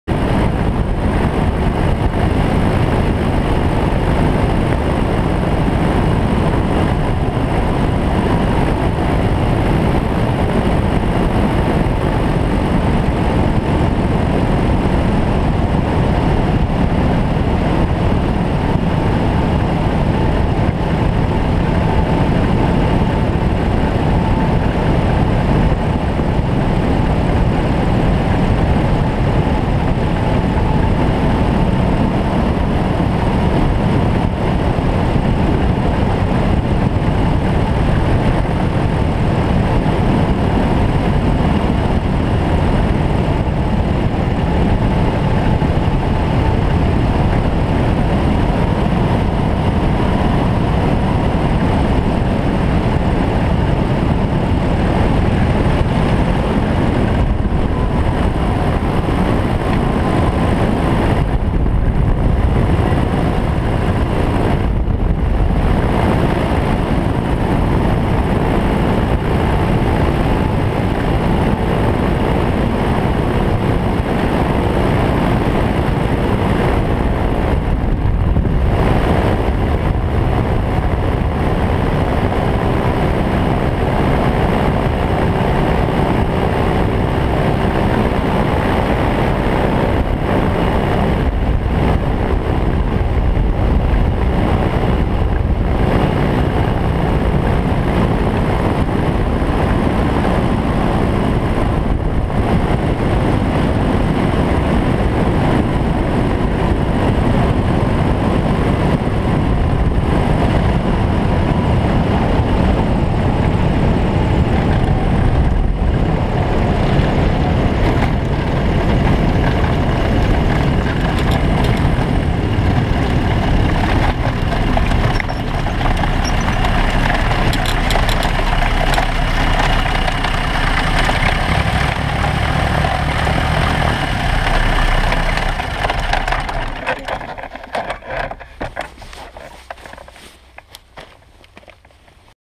3rd Flight Bautek Skycruiser, landing UL-Metelen in Germany
My 3rd flight, now from Lelystad Airport the Netherlands (EHLE) to microlight landing strip UL-Metelen in Germany (appr. 130km one way). Arrival in Metelen after 1 hour and 40 minutes. This video shows only the landing in Metelen. Not by the book, sorry for that, especially flying over the nearby farm. Next time I will fly a circuit. There was not much traffic. I flew over to see if there was any wind and then landed, but I should have flown a right hand circuit!
Nice people there and a good Microavionics retailer.